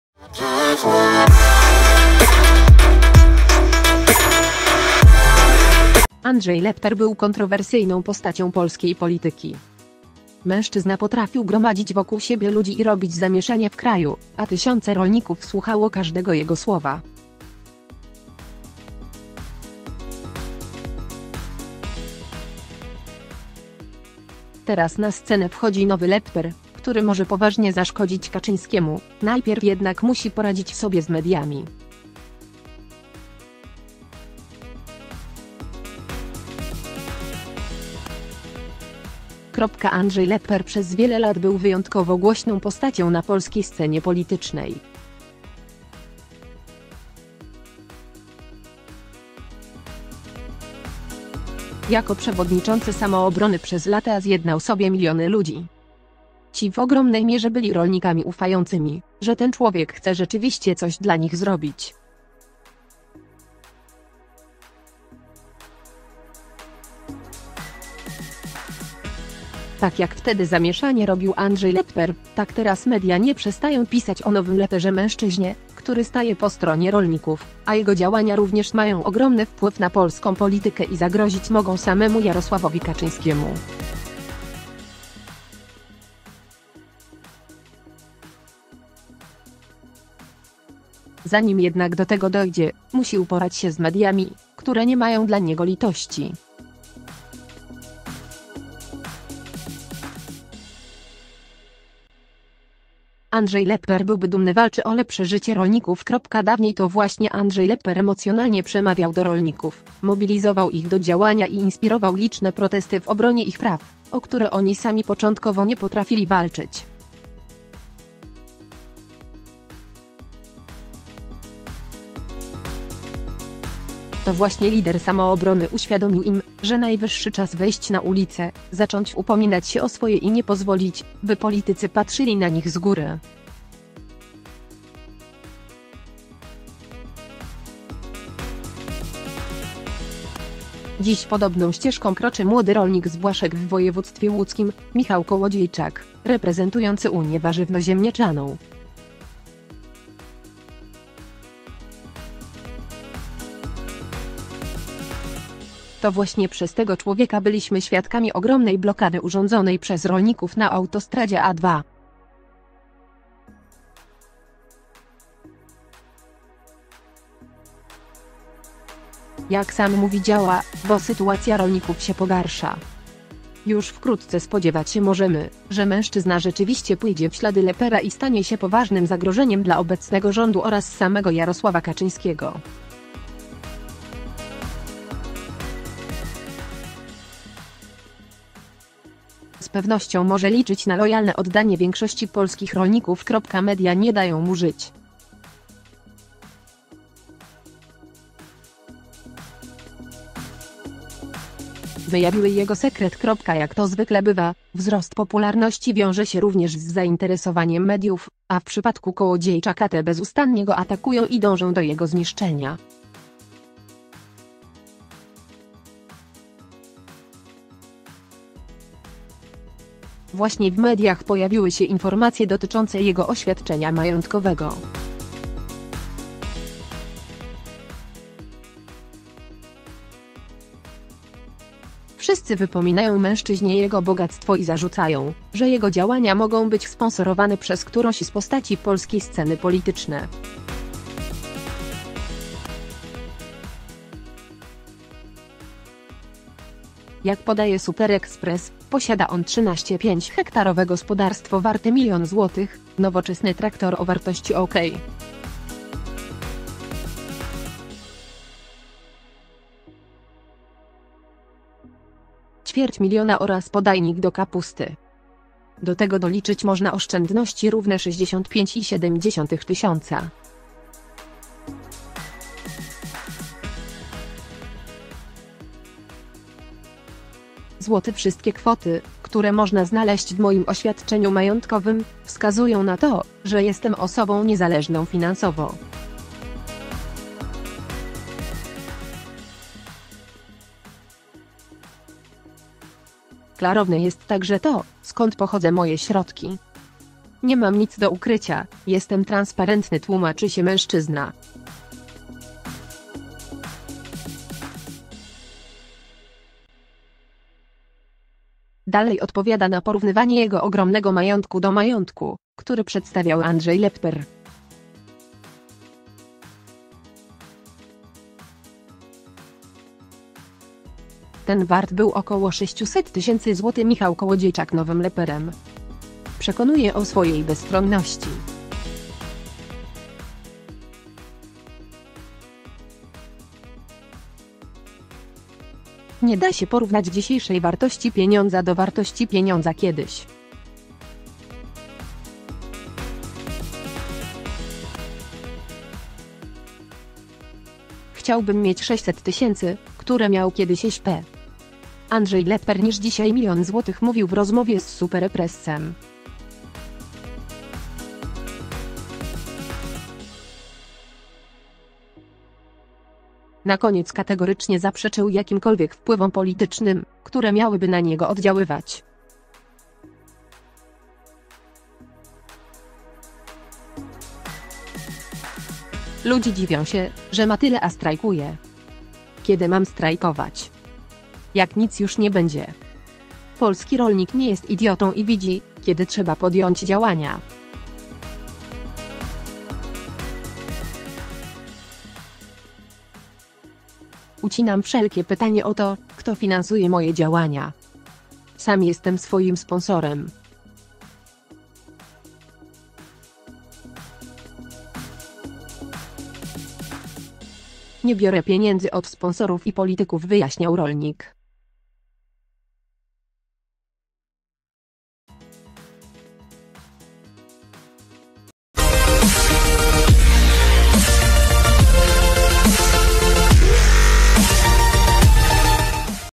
Nowy Lepper przeraził Kaczyńskiego?! Zaczęło się, media nie mają dla niego litości | Aktualności 360
Andrzej Lepper był kontrowersyjną postacią polskiej polityki. Mężczyzna potrafił gromadzić wokół siebie ludzi i robić zamieszanie w kraju, a tysiące rolników słuchało każdego jego słowa. Teraz na scenę wchodzi „nowy Lepper”, który może poważnie zaszkodzić Kaczyńskiemu, najpierw jednak musi poradzić sobie z mediami.
Andrzej Lepper przez wiele lat był wyjątkowo głośną postacią na polskiej scenie politycznej. Jako przewodniczący Samoobrony przez lata zjednał sobie miliony ludzi. Ci w ogromnej mierze byli rolnikami ufającymi, że ten człowiek chce rzeczywiście coś dla nich zrobić.
Tak jak wtedy zamieszanie robił Andrzej Lepper, tak teraz media nie przestają pisać o „nowym Lepperze” – mężczyźnie, który staje po stronie rolników, a jego działania również mają ogromny wpływ na polską politykę i zagrozić mogą samemu Jarosławowi Kaczyńskiemu. Zanim jednak do tego dojdzie, musi uporać się z mediami, które nie mają dla niego litości.
Andrzej Lepper byłby dumny – walczy o lepsze życie rolników
Dawniej to właśnie Andrzej Lepper emocjonalnie przemawiał do rolników, mobilizował ich do działania i inspirował liczne protesty w obronie ich praw, o które oni sami początkowo nie potrafili walczyć. To właśnie lider Samoobrony uświadomił im, że najwyższy czas wyjść na ulice, zacząć upominać się o swoje i nie pozwolić, by politycy patrzyli na nich z góry.
Dziś podobną ścieżką kroczy młody rolnik z Błaszek w województwie łódzkim, Michał Kołodziejczak, reprezentujący Unię Warzywno-Ziemniaczaną. To właśnie przez tego człowieka byliśmy świadkami ogromnej blokady urządzonej przez rolników na autostradzie A2.
Jak sam mówi „działa, bo sytuacja rolników się pogarsza”. Już wkrótce spodziewać się możemy, że mężczyzna rzeczywiście pójdzie w ślady Leppera i stanie się poważnym zagrożeniem dla obecnego rządu oraz samego Jarosława Kaczyńskiego. Z pewnością może liczyć na lojalne oddanie większości polskich rolników.
Media nie dają mu żyć. Wyjawiły jego sekret
Jak to zwykle bywa, wzrost popularności wiąże się również z zainteresowaniem mediów, a w przypadku Kołodziejczaka te bezustannie go atakują i dążą do jego zniszczenia. Właśnie w mediach pojawiły się informacje dotyczące jego oświadczenia majątkowego. Wszyscy wypominają mężczyźnie jego bogactwo i zarzucają, że jego działania mogą być sponsorowane przez którąś z postaci polskiej sceny polityczne.
Jak podaje „Super Express”, posiada on 13,5-hektarowe gospodarstwo warte milion złotych, nowoczesny traktor o wartości ok. ćwierć miliona oraz podajnik do kapusty. Do tego doliczyć można oszczędności równe 65,7 tys. zł.
– Wszystkie kwoty, które można znaleźć w moim oświadczeniu majątkowym, wskazują na to, że jestem osobą niezależną finansowo. Klarowne jest także to, skąd pochodzę moje środki. Nie mam nic do ukrycia, jestem transparentny – tłumaczy się mężczyzna.
Dalej odpowiada na porównywanie jego ogromnego majątku do majątku, który przedstawiał Andrzej Lepper. Ten wart był około 600 tys. zł.
Michał Kołodziejczak „nowym Lepperem”. Przek